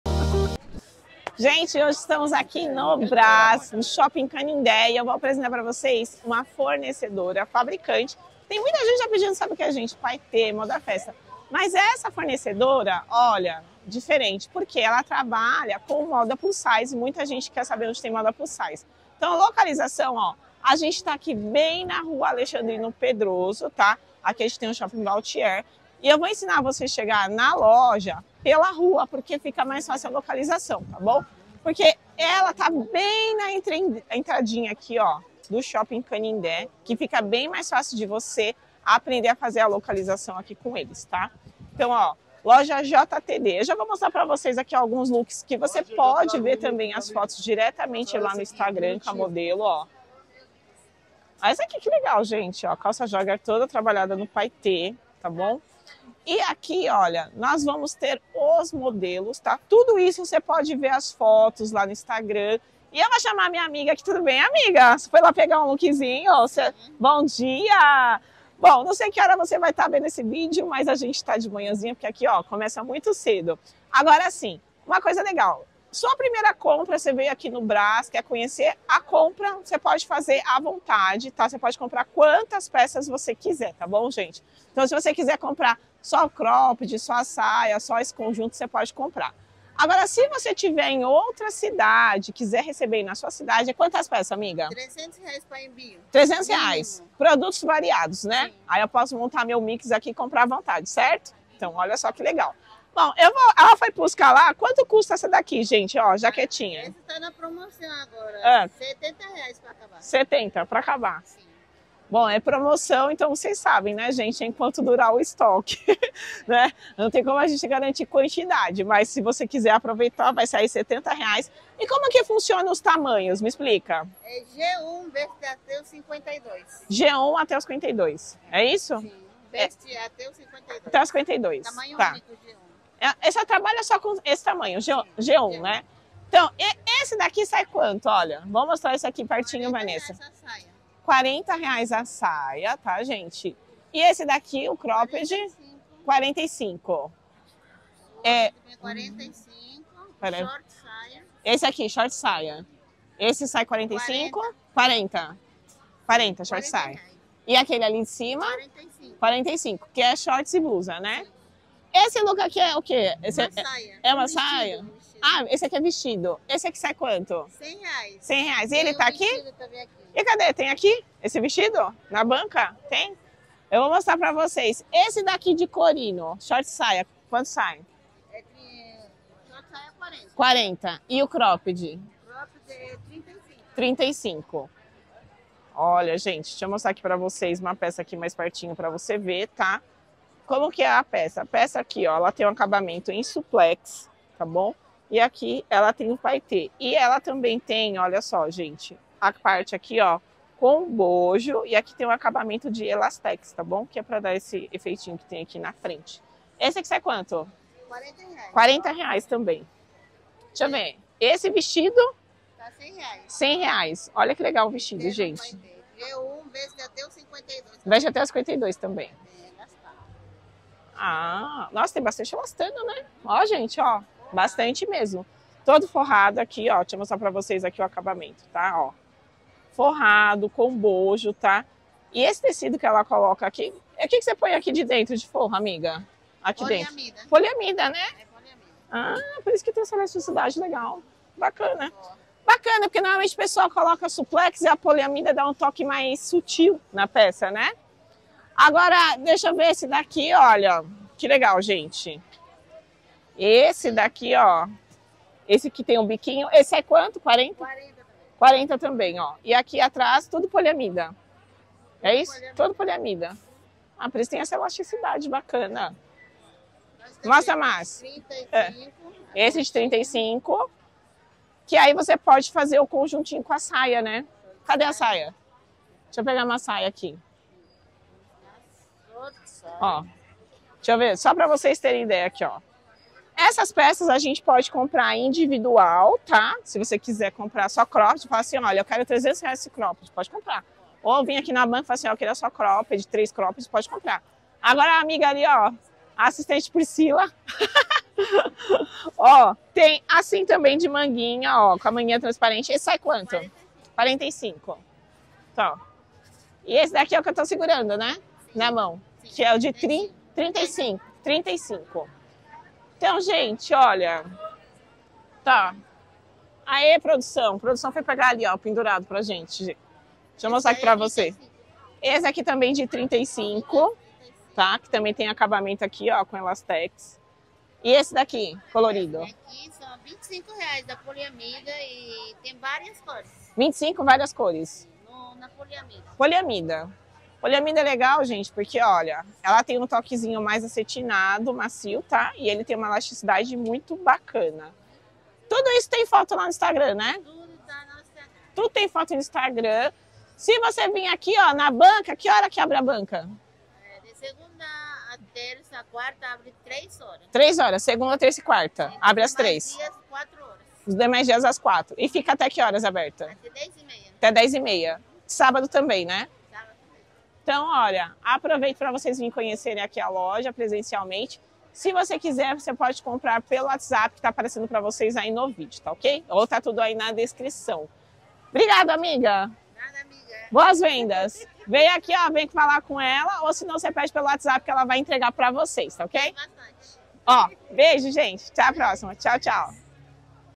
FASHION PLUS SIZE: ENCONTRE SEU ESTILO
No video de hoje vamos mostrar a moda que muito de vocês pedem, a moda plus size.
 Localizada no shopping canindé, rua Tiers 310, banca 656-666.
A negociação entre cliente e fornecedor é de total
responsabilidade dos mesmos.
Os preços mostrados valem para a época do
lançamento do vídeo.